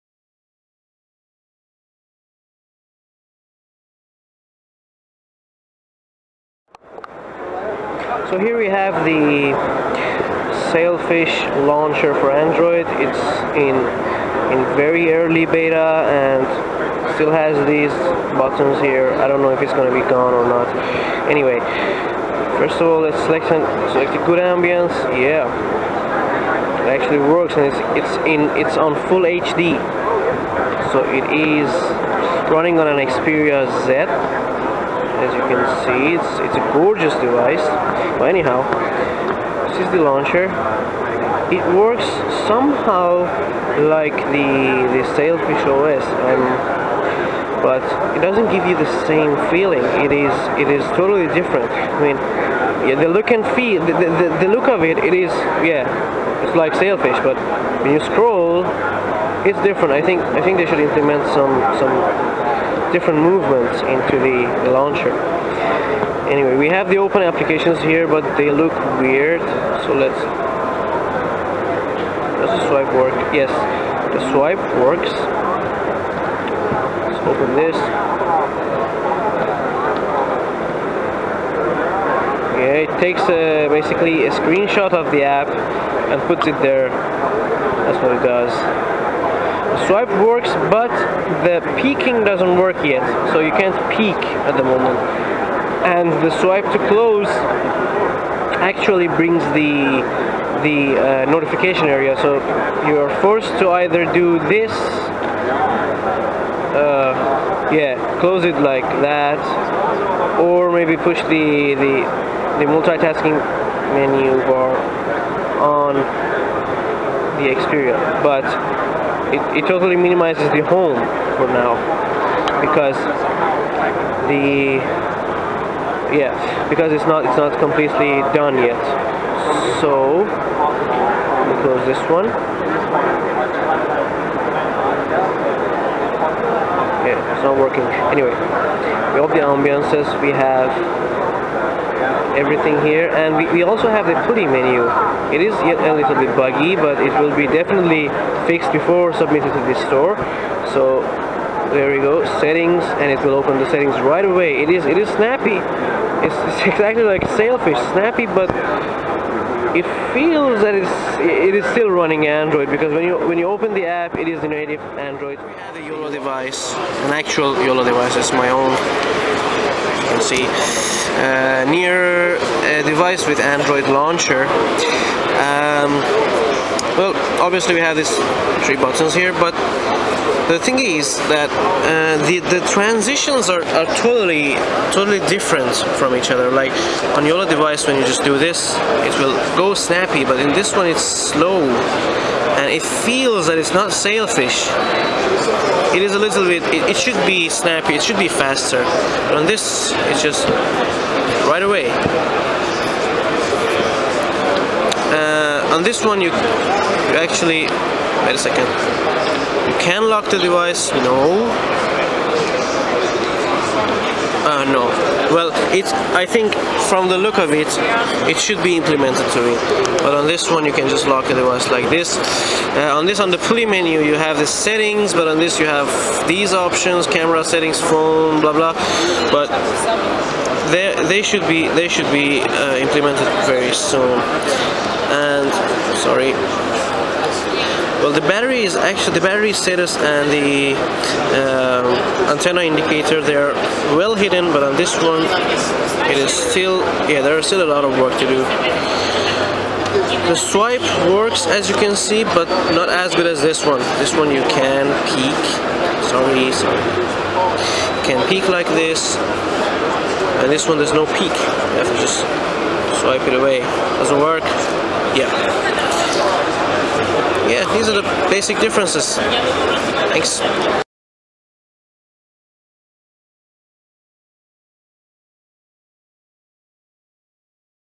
Sailfish Launcher for Android Preview
Check out the beta version of SailfishOS Android Launcher demoed in comparison to the real Sailfish OS.

-This is a launcher not the OS itself
-This is only a pre-beta program that I got permission to record it
-The launcher has bugs so bear with Jolla until they release an actual proper one to Google play
-I apologize about the focus, the camera was playing hide and seek with me and the time was tight.

There definitely is a lot of work to be done obviously but hey we're slowly getting somewhere ;)